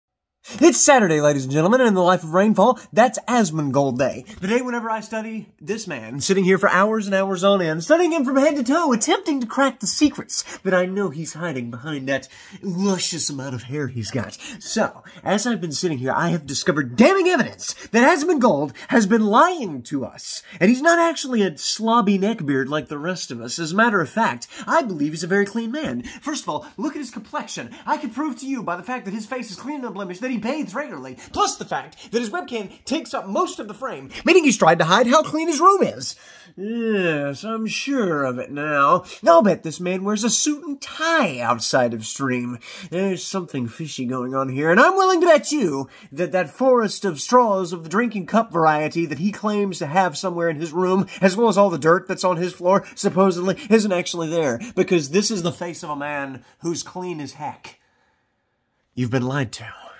Asmongold is lying to you about his lifestyle
He's just PRETENDING to be a neckbeard. In reality, I think he's hiding the fact that he vacuums his bedroom regularly.